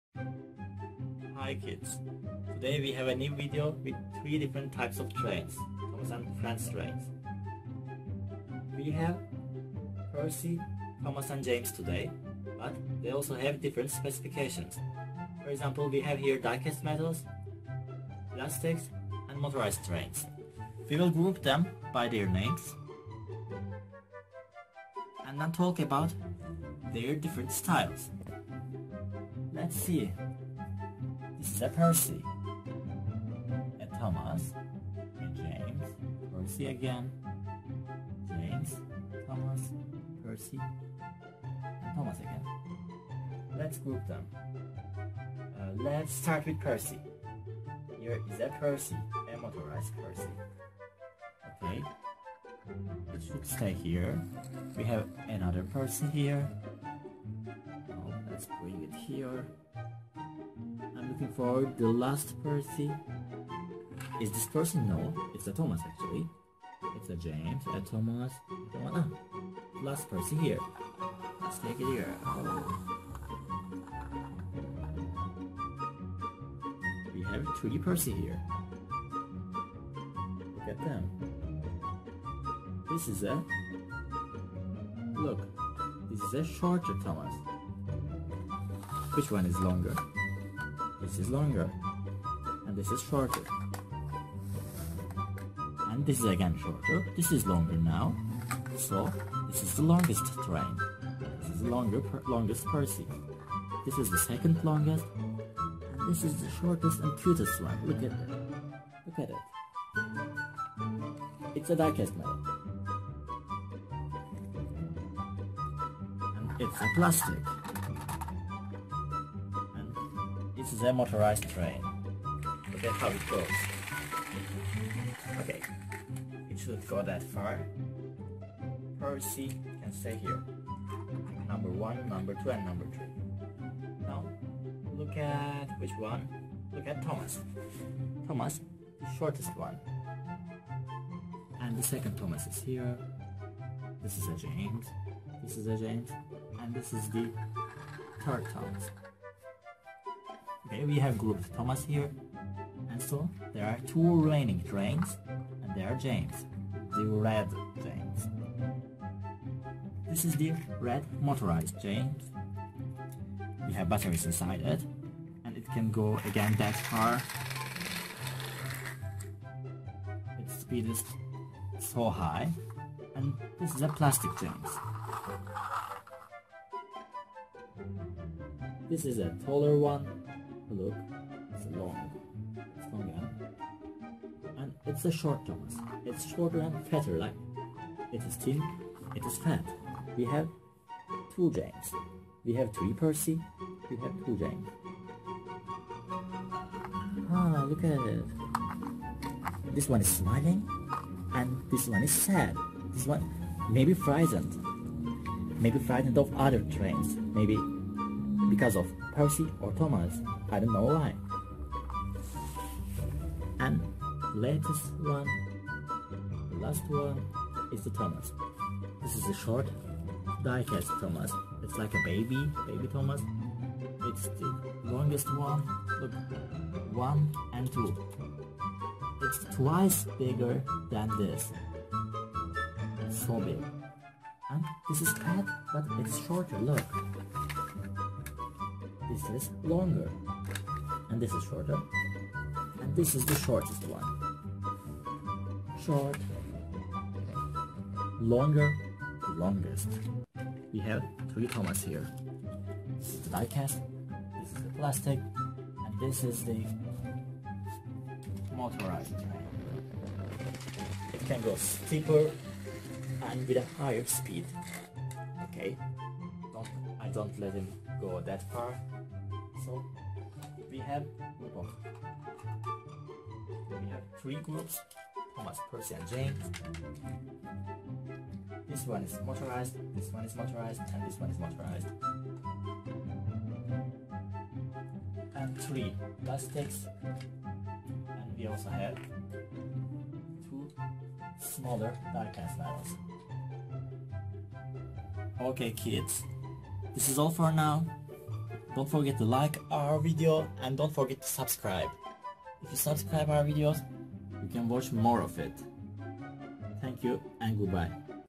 Thomas and Friends Toys Learn Size with Thomas Percy James Trains
This YouTube video episode is about Thomas and Friends -  Learn Size with Thomas Percy James Trains Toys